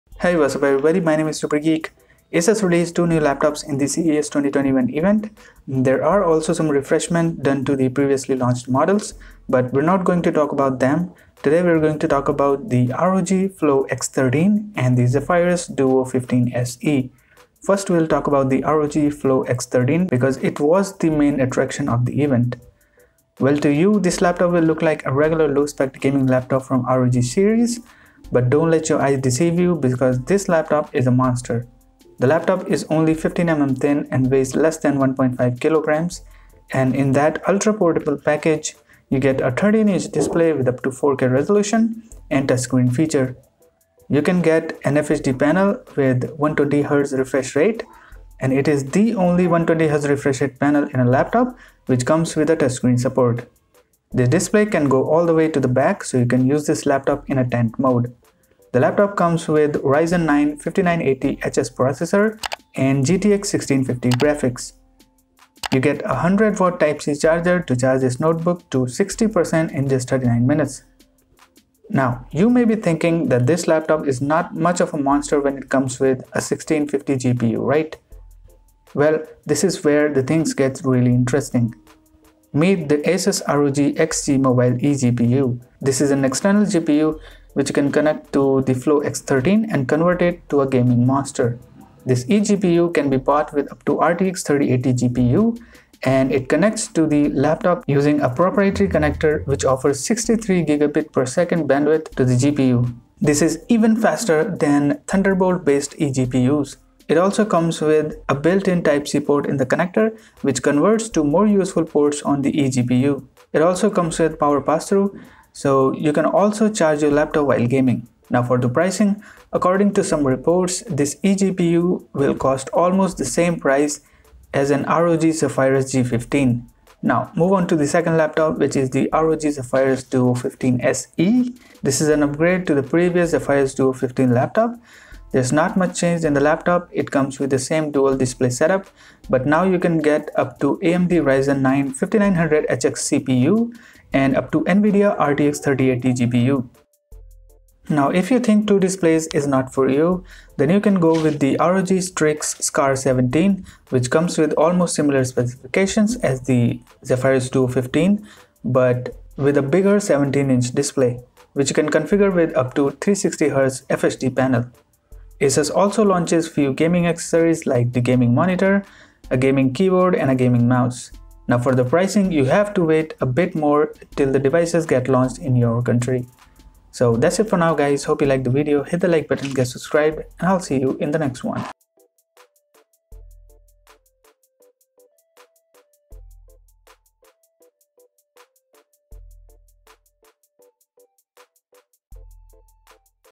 Asus ROG Flow X13 Launched with 3 GPUs, CES 2021
Asus released its own Razer Blade Pro rival which utilizes an external GPU to enhance gaming.